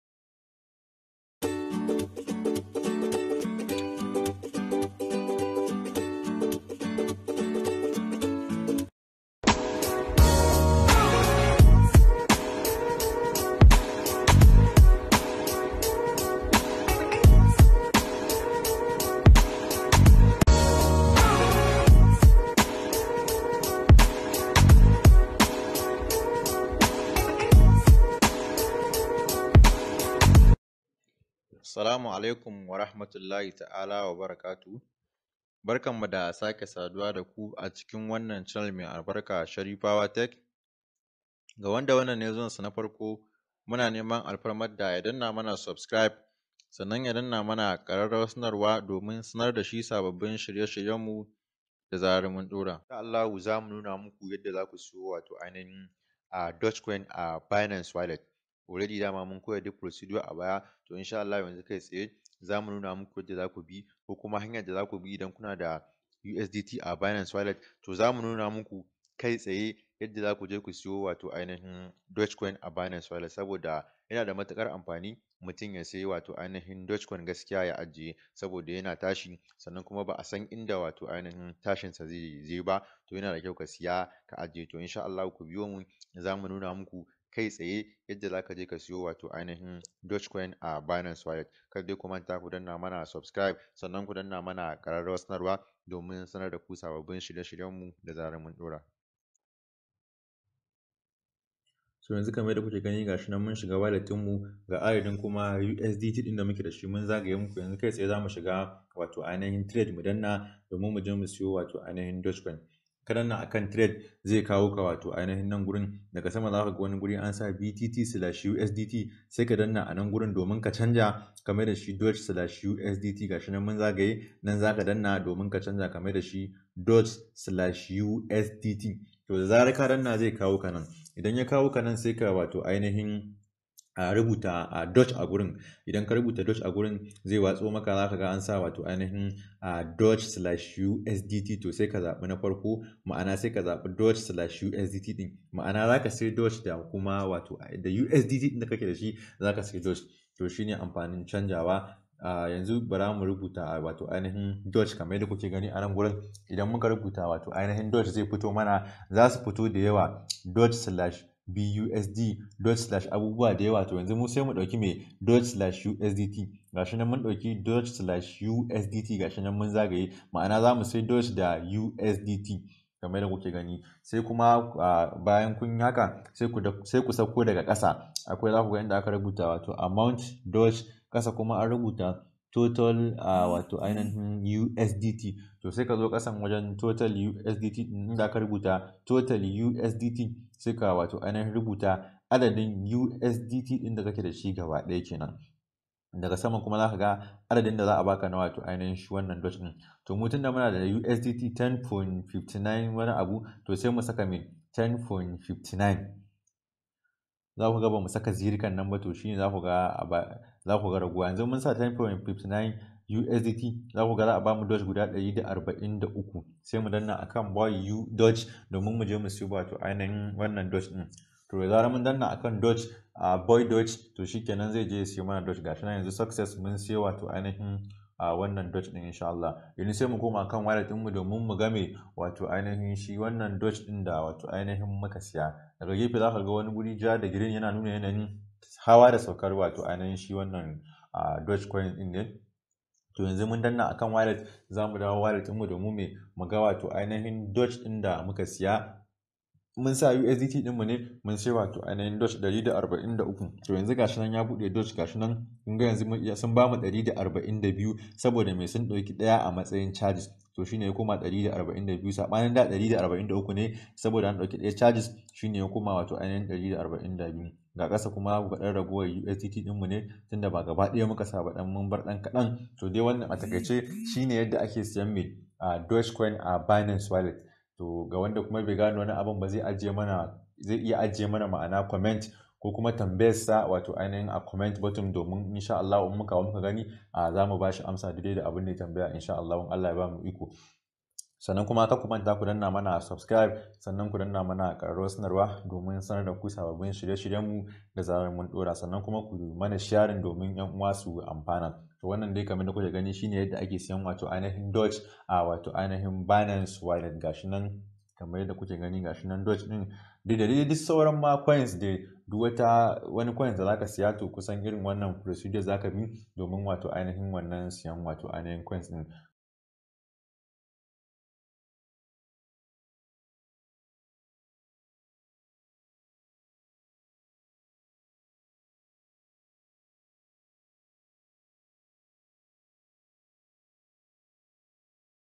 HOW TO BUY DOGE COIN ON BINANCE
In this video i will show you step by step how to buy dogecoin in binance